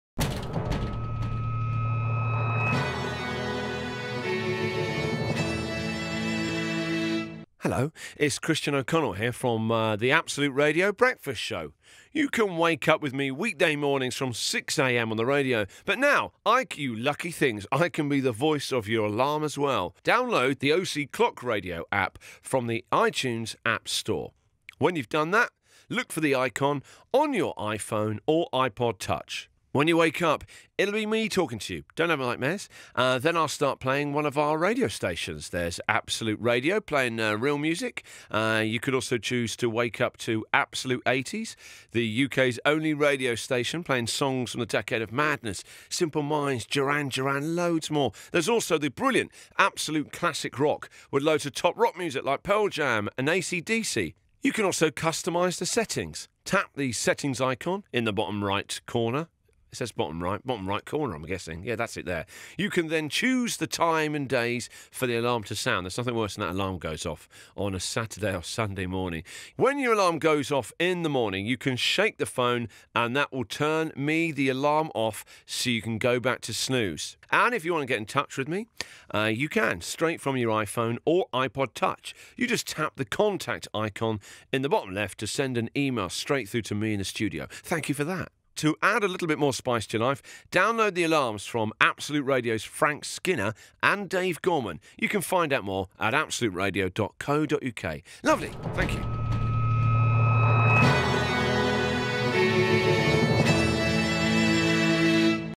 THE OC CLOCK RADIO ALARM - DEMONSTRATION
Now Absolute Radio's Christian O'Connell can wake you up even when he's not on-air thanks to this new app available from iTunes.